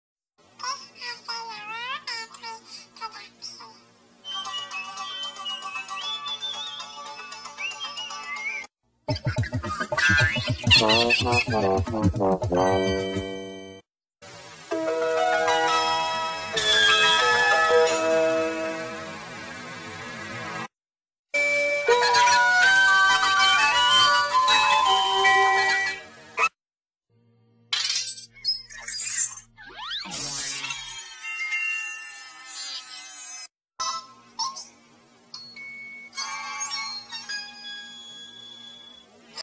(REQUESTED) Noggin and Nick Jr. Logo Collection in Rome Major 1
Requested by TheHarpreet2024 Teh Reuploader EDCP ATP.

Effect by myself, effect made on April 27, 2019